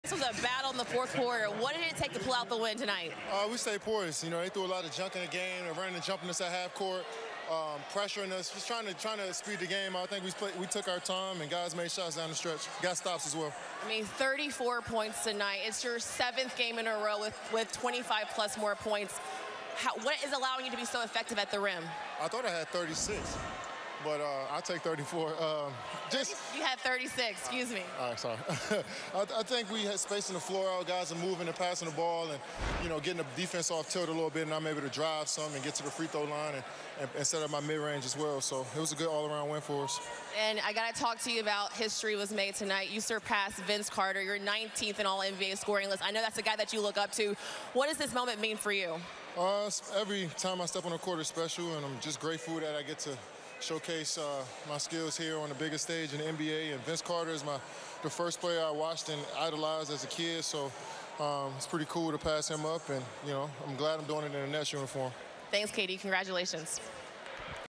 Kevin Durant after the Nets' win over the Pacers
NETS WIN❕Meghan Triplett collects Kevin Durant’s thoughts after passing Vince Carter as the 19th all-time scorer.